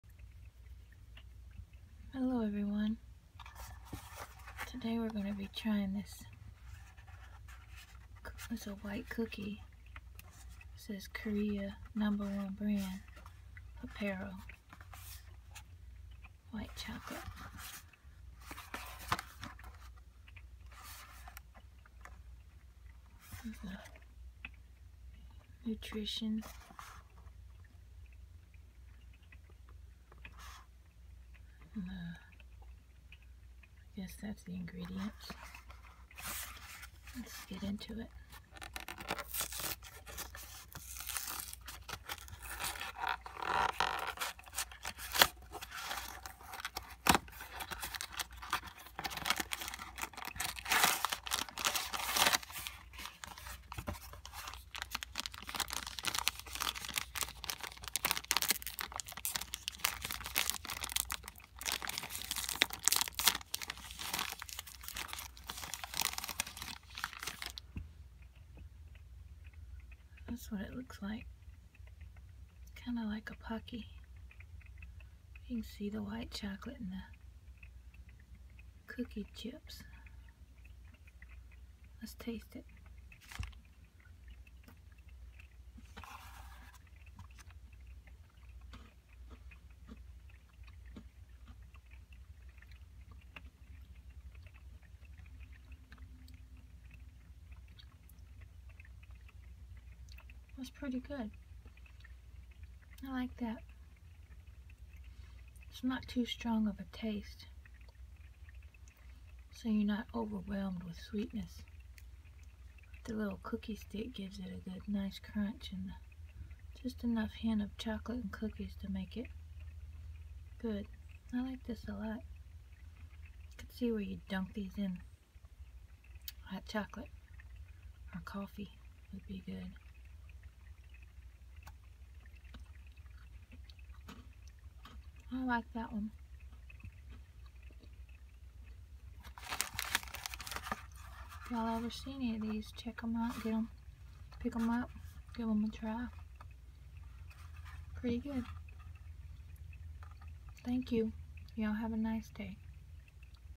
Korean Pepero Tasting ASMR
This is a really good snack from Korea. Try it out of you see any.